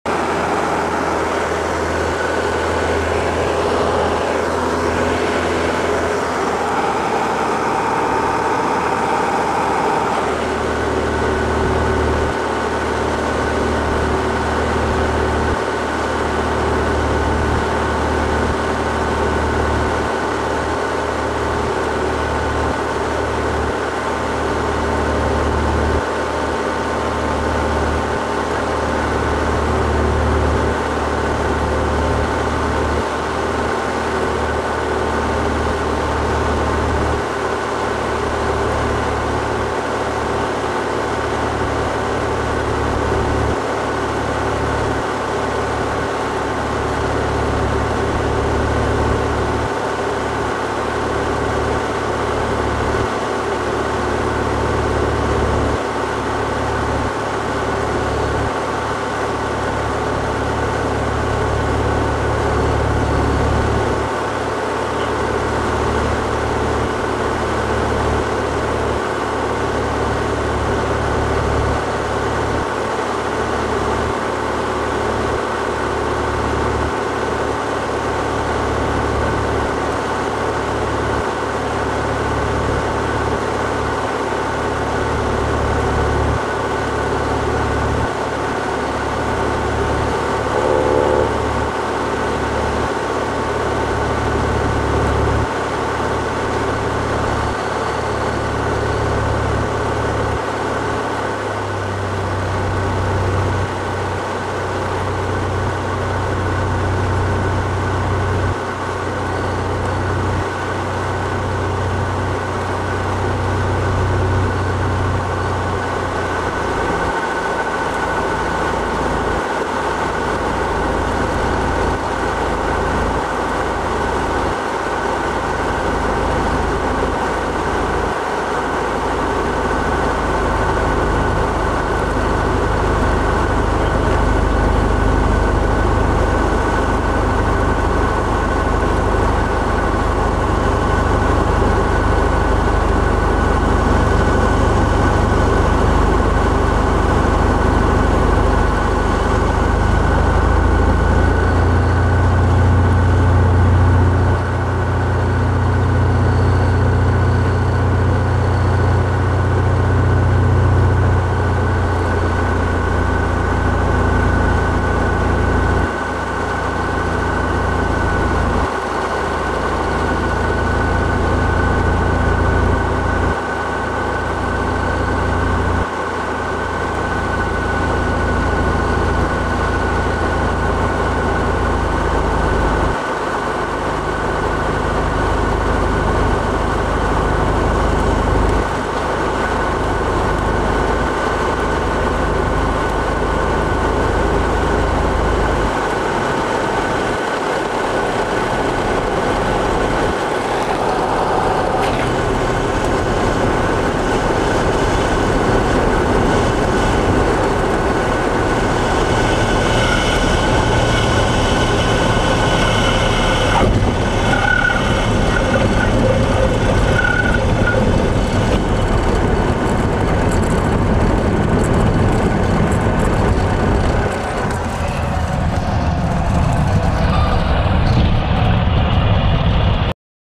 Prince Edward Island: landing a Cessna at Slemon Park Airport near Summerside
Returning from taking aerial photos and aerial videos; Michael Poczynek, Waterfront & Development Specialist since 1997 of Century 21 Northumberland Real Estate in Summerside, Prince Edward Island, is a passenger in a Cessna 172 landing at Slemon Park Airport just north-west of Summerside, PEI.   More information about Michael can be found Custom aerial photos/videos and tours of PEI can be arranged.